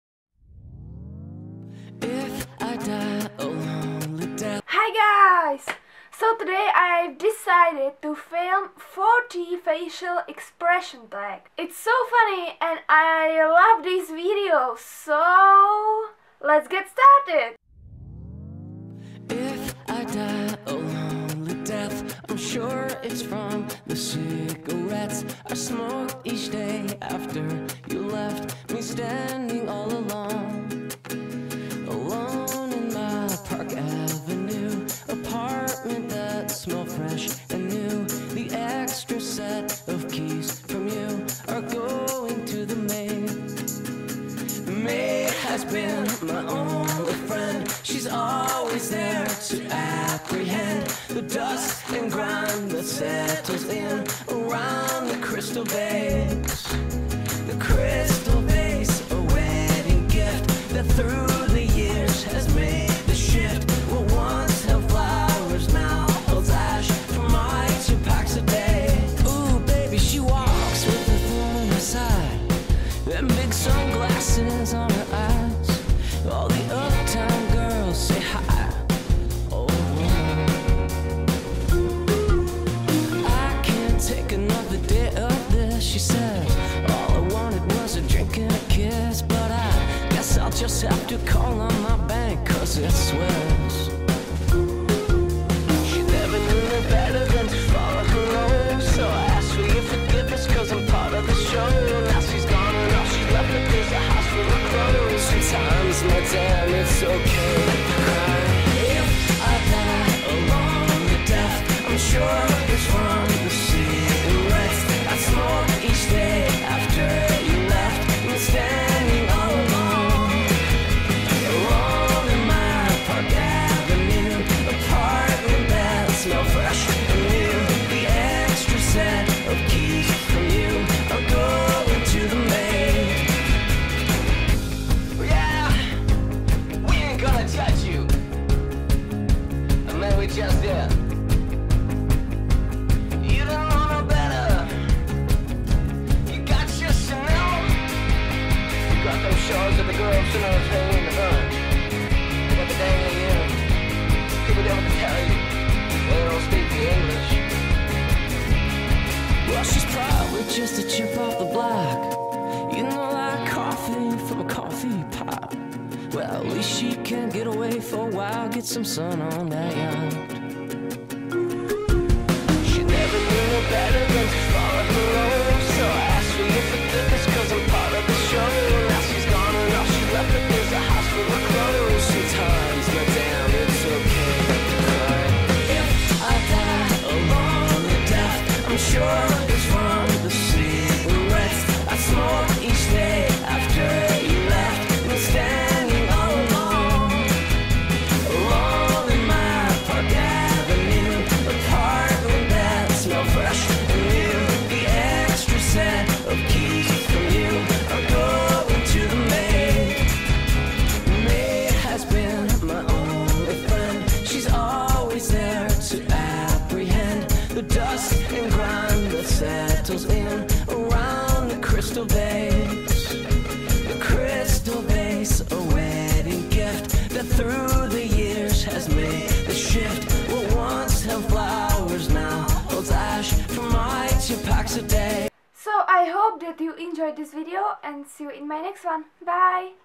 40 Facial Expressions TAG
I love these videos ♥

1.Happy
2.Sad
3.Scared
4.Stressed
5.Shy
6.Shocked
7.Innocent
8.Unattractive
9.Asian pose
10.Sarcastic
11.In love
12.Huffy
13.Threatening
14.Typical teenager
15.Annoyed
16.Insulted
17.Lost
18.Pissed off
19.Flirty
20.Tired
21.Bitchy
22.Listening to music
23.Aggravated by a fly
24.Im watching you
25.Evil
26.Anxious
27.Smart
28.Crying
29.Checking someone out
30.Outcast
31.Bored
32.Daydreaming
33.Refreshed ( I completely forgot )
34.Cold
35.Hot
36.Fake
37.Excited
38.Falling asleep
39.Confused
40.Winking